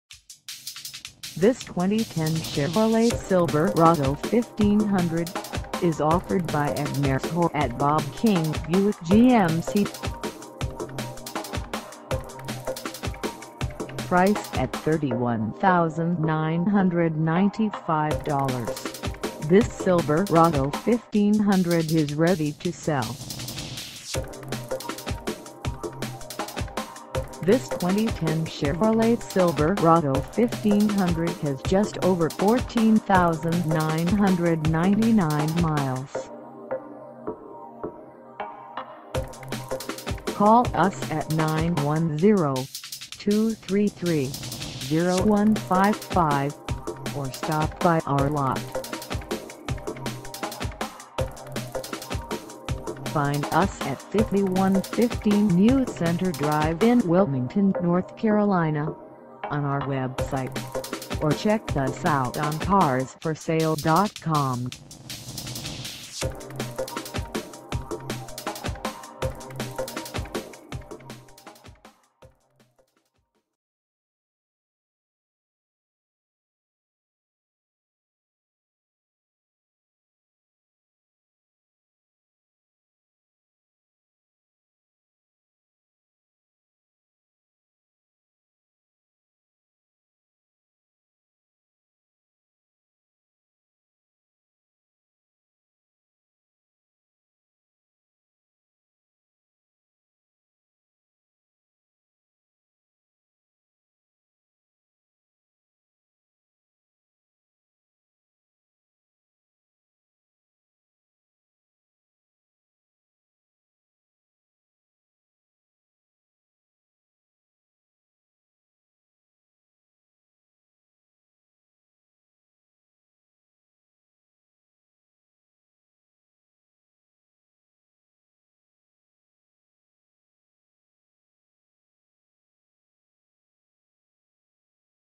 2010 Chevrolet Silverado 1500 LTZ - for sale in Wilmington, NC 28403
Ed Meshaw at Bob King Buick GMC
5115 NEW CENTRE DR

Come test dirve this 2010 Chevrolet Silverado 1500 LTZ for sale in Wilmington, NC.

Convenience Package (Power-Adjustable Pedals For Accelerator & Brake), GM Certified, 6-Speed Automatic, 4WD, 6'' Oval Chromed Tubular Assist Steps, EZ-Lift Tailgate, LTZ Plus Package, and Rear-View Camera System. Want to stretch your purchasing power? Well take a look at this rock solid 2010 Chevrolet Silverado 1500. Buying a truck with numerous previous owners can be a little risky, but no worries here on this wonderful one-owner truck. New Car Test Drive said, ''...a smooth ride and confident handling while delivering superior capability and power...among the smoothest riding and quietest of all full-size pickups, and can be counted on to get the job done...'' Designated by Consumer Guide as a 2010 Recommended Large Pickup. GM Certified Pre-Owned means you not only get the reassurance of a 12mo/12,000-Mile Bumper-to-Bumper limited warranty, but also a 2yr/30,000-Mile Standard CPO Maintenance Plan, up to a 5-Year/100,000-Mile, $0 Deductible, Fully Transferable, Powertrain Limited Warranty, a 172-point inspection/reconditioning, 24/7 roadside assistance, Courtesy Transportation, 3-Day/150-Mile Customer Satisfaction Guarantee, 3 month trial OnStar Directions & Connections Service if available, SiriusXM Radio 3mo trial if available, and a complete CARFAX vehicle history report.